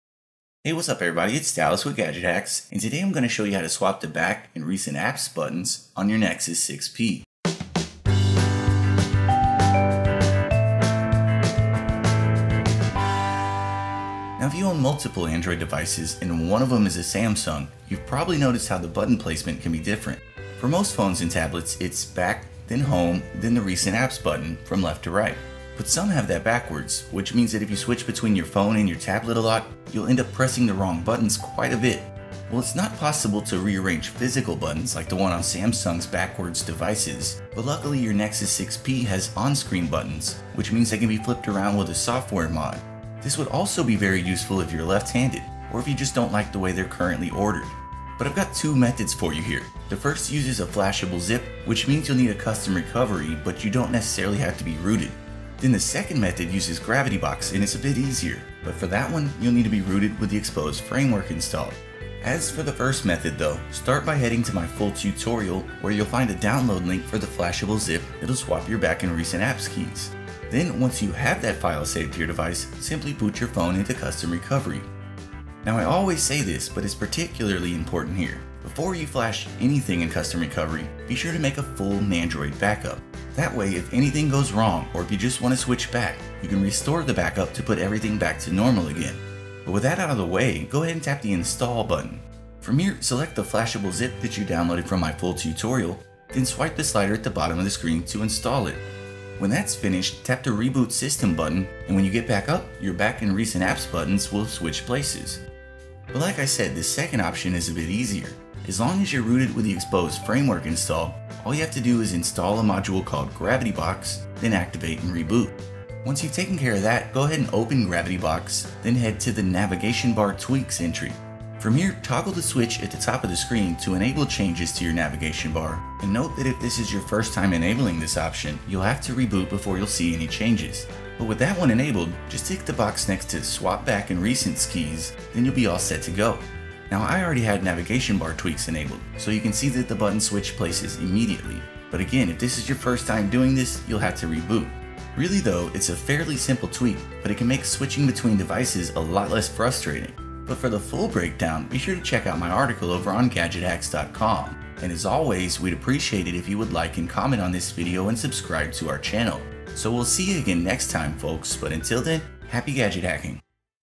Swap the 'Back' & 'Recent Apps' Keys on Your Nexus 6P [How-To]
How Change the Layout of Buttons on the Nexus 6P [Root]

In this video, I'll be showing you two methods you can use to swap the placement of the "Back" and "Recent Apps" buttons on your Nexus 6P. The first is a flashable ZIP, which can be installed with custom recovery.

The second is an option in GravityBox, which is an Xposed module, so you'll need to be rooted with the Xposed Framework installed for that one.

Either way you do it, swapping these buttons out can be helpful. If you're left-handed, it's a more natural layout. Or, if you own multiple Android devices and one of them is a Samsung with its backwards buttons, this can make switching between them a more consistent experience.